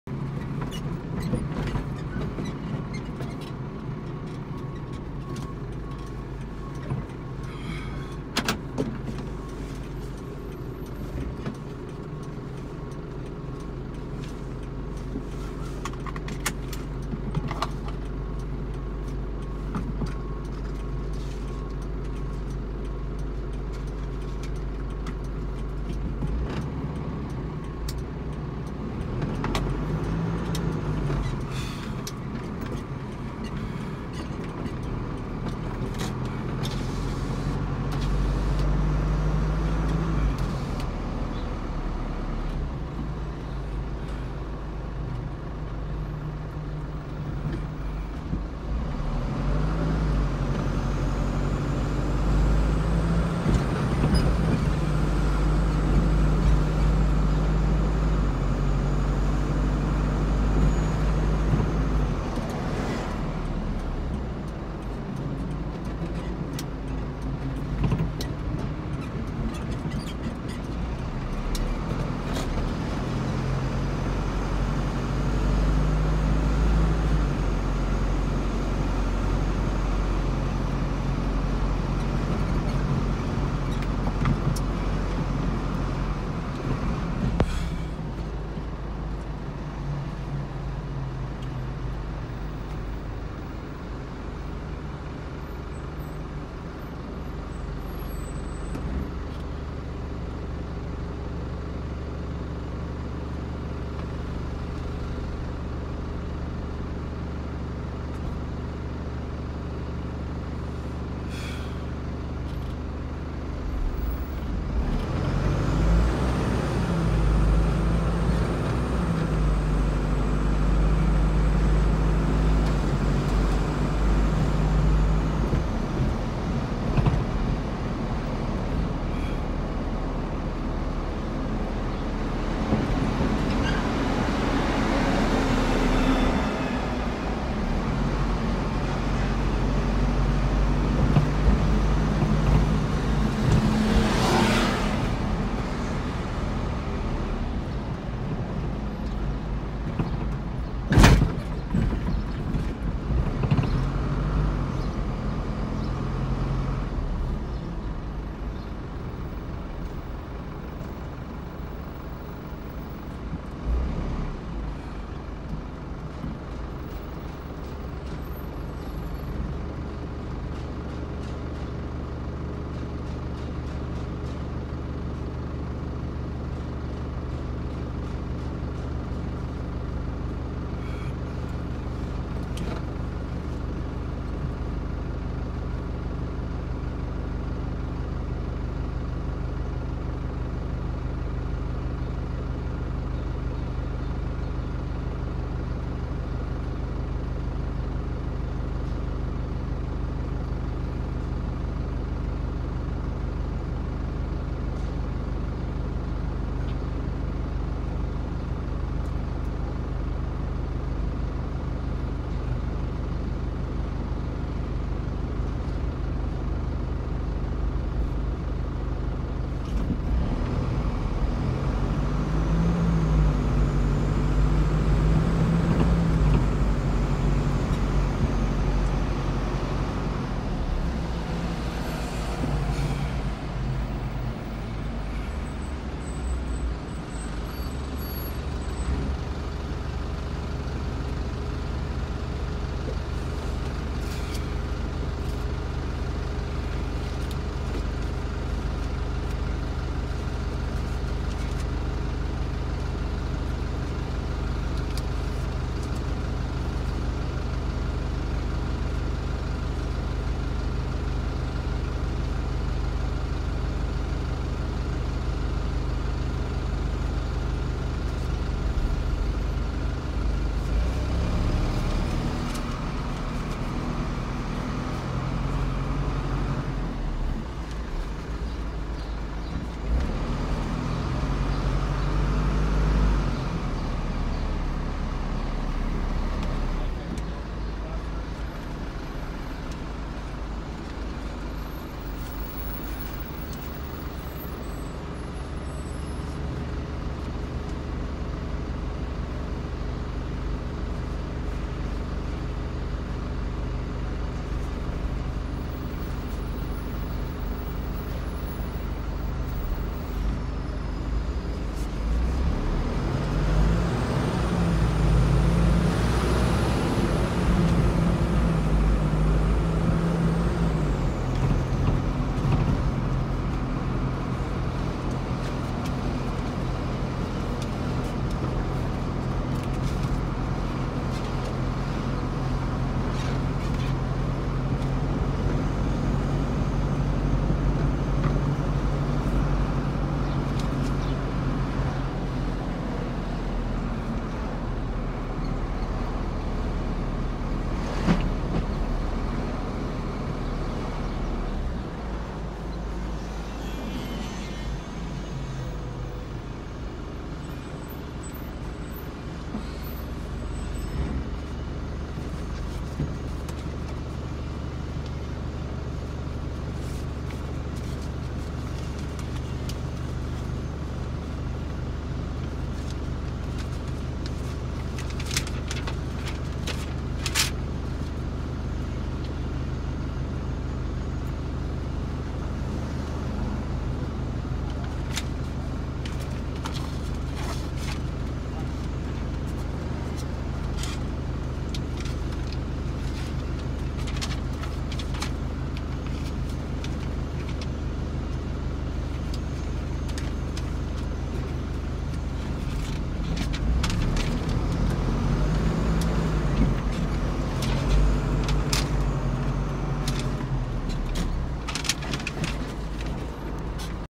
Driving through Downtown MTL during a pandemic
Ghost town Downtown Montreal 2021